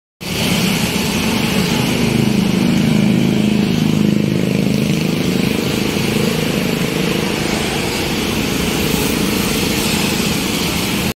FITS AIR AIRBUS A320
Fits Air(8D833) AIRBUS A320 Aircraft landing @ Runway 25 of Chennai Airport from Colombo (Sri Lanka) on late Sunday Afternoon